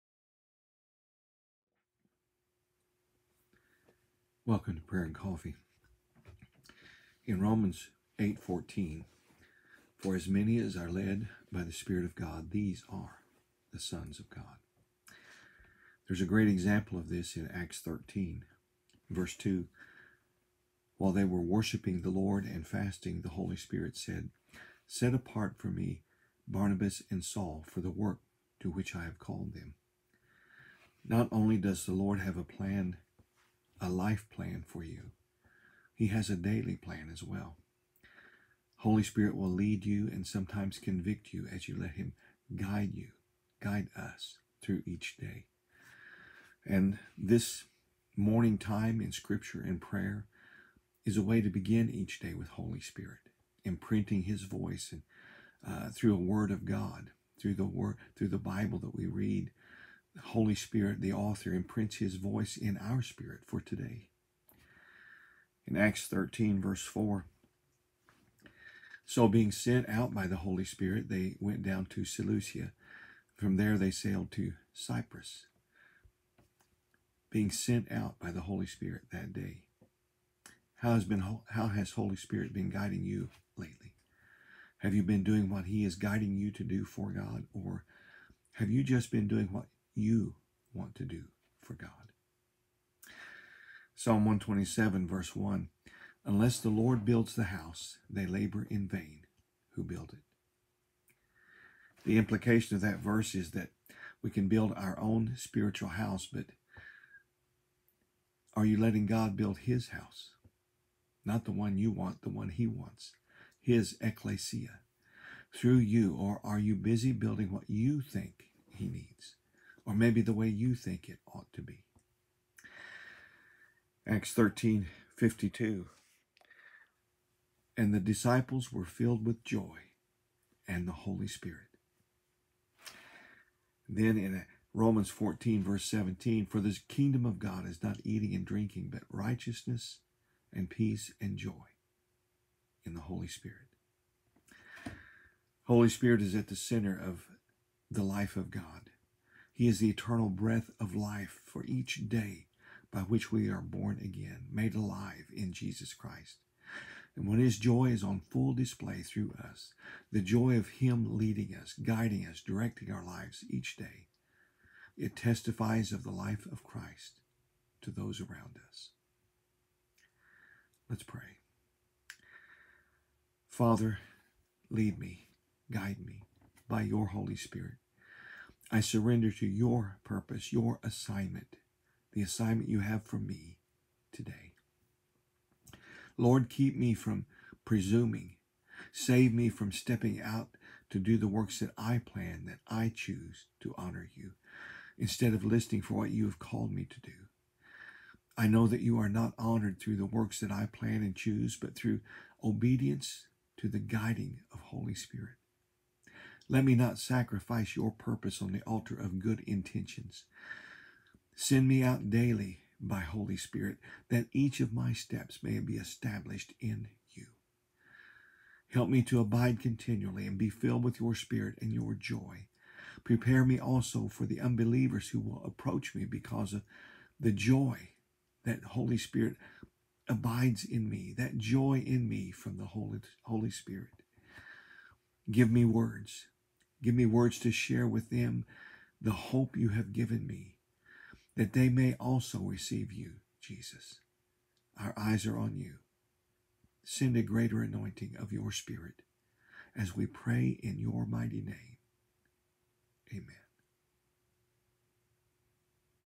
Prayer & Coffee... Saturday, May 23, 2020
Not only does the Lord have a life-plan for you, He has a daily plan as well. Holy Spirit will lead you, as you let Him, through each day…